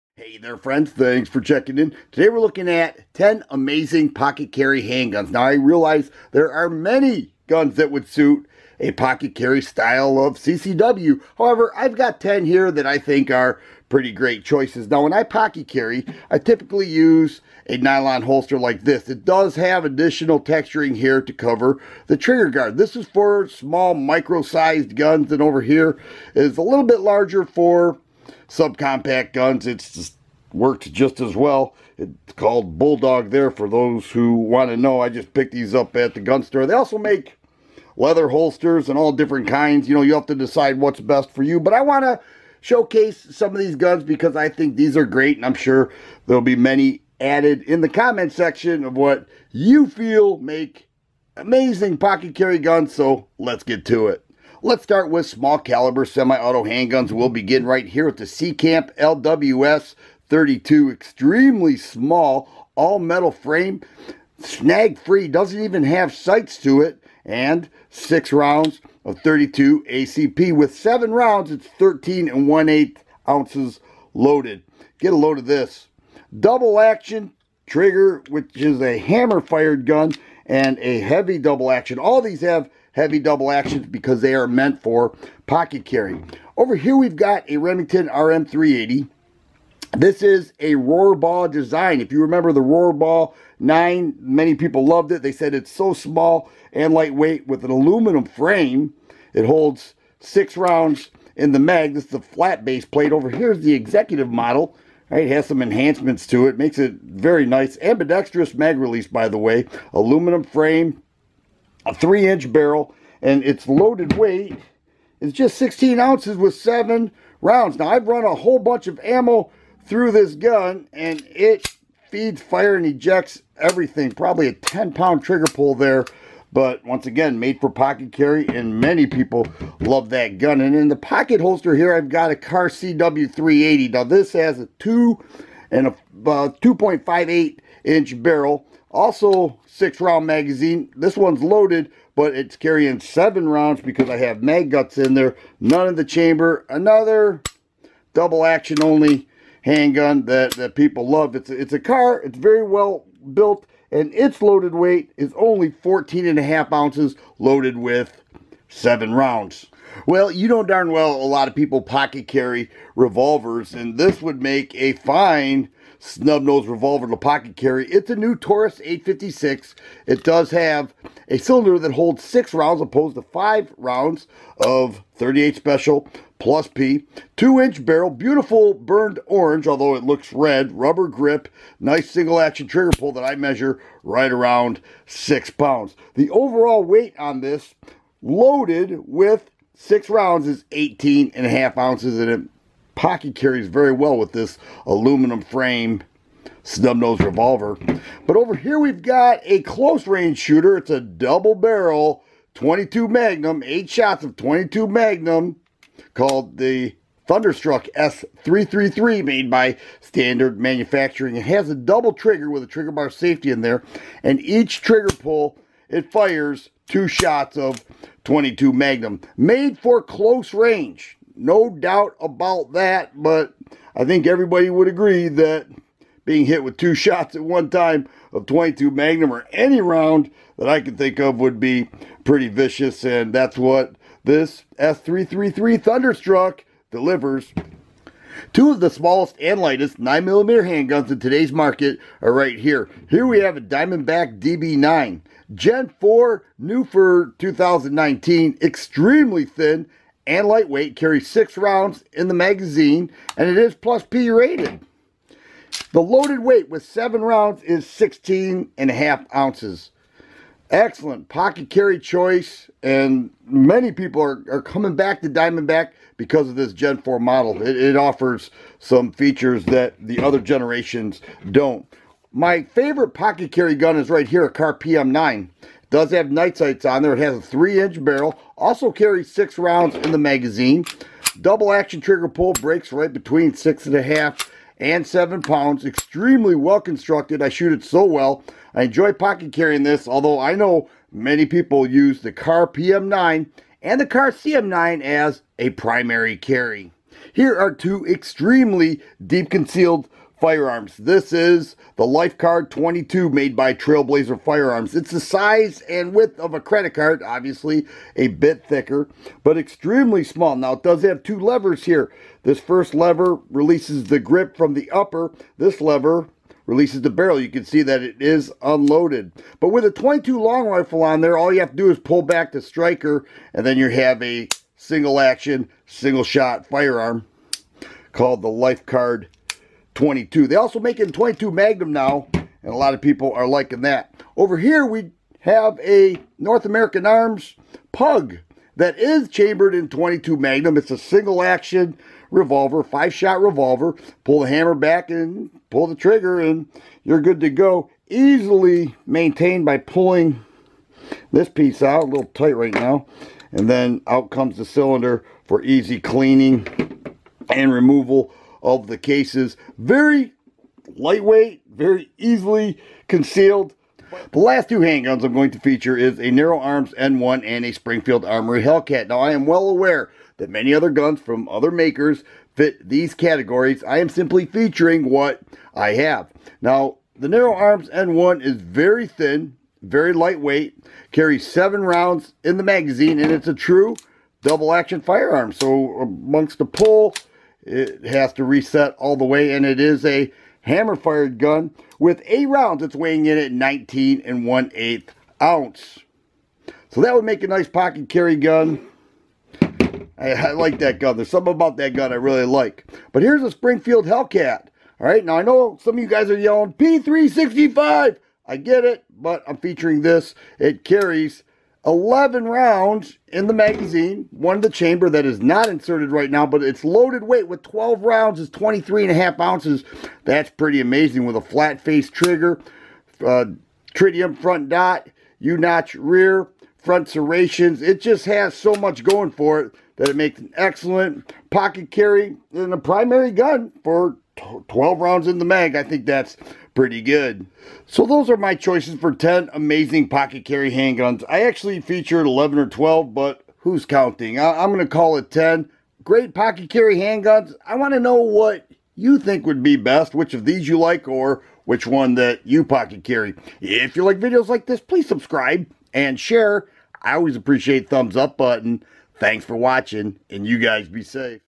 10 Amazing "Pocket Carry" Handguns - TheFirearmGuy
Here are 10 amazing "Pocket Carry" Handguns. Do you like these handguns or do you have a better pistol for pocket carry? Let me know you thoughts and which of the handguns featured you have experience with.